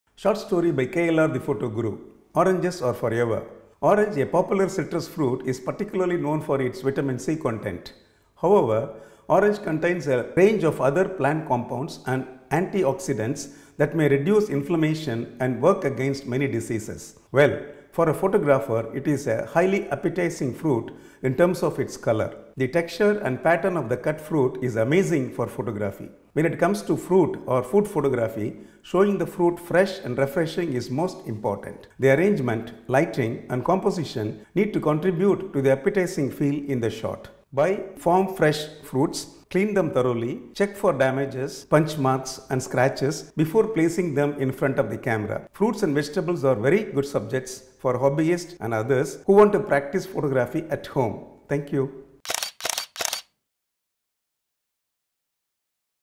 ShotStori by KLR the photo guru - Oranges are forever
Orange popular citrus fruit is particularly known for its vitamin C content. However, oranges contain a range of other plant compounds and antioxidants that may reduce inflammation and work against many diseases.

Well, for a photographer, it is a highly appetizing fruit in terms of its colour! The texture and pattern of cut fruit are amazing for photography. When it comes to fruit (food) photography, showing the fruit fresh and refreshing is most important. The arrangement, lighting, and composition need to contribute to the appetizing feel in the shot. Please check all these in the shot and pick up some points.
Bottom line: Fruits and vegetables are very good subjects for hobbyists and others who want to practice photography at home. A subject you can eat after the shoot!
Learn to see the world around series

Watch ‘KLR the photo guru’ my YouTube channel for more tutorials on photography.